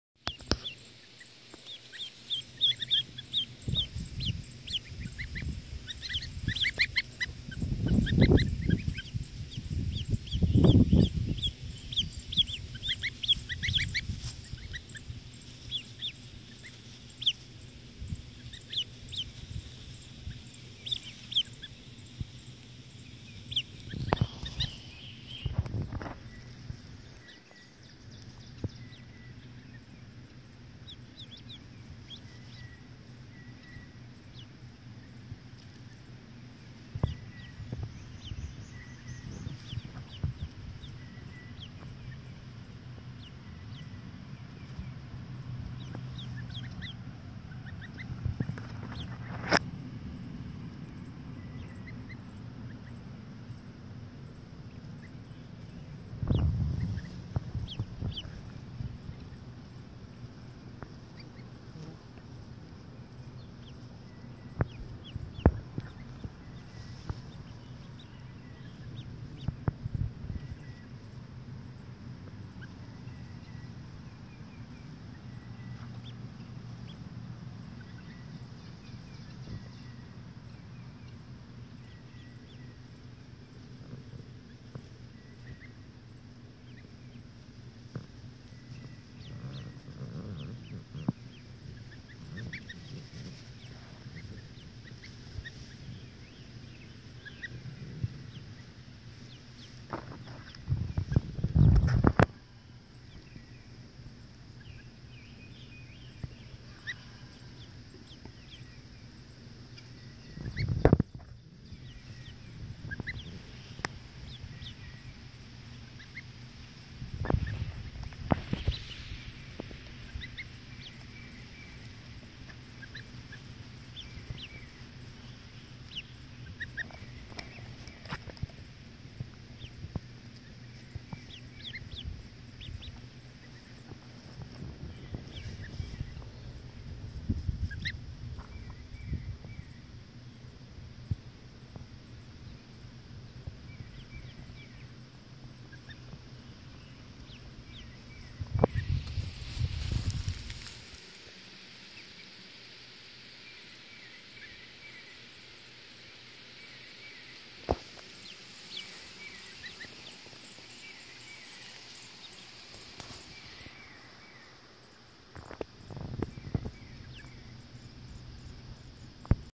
Freshly hatched babies having a little "salad"
I have a brooder FULL of baby waterfowl right now.  I like to get the goslings out for a little salad every evening and tonight I have three little goose babies.  I gathered them and a few little ducklings that were snuggled close to those fat gooses for everyone to get a little browse in the clover. Tonight was the perfect evening for it!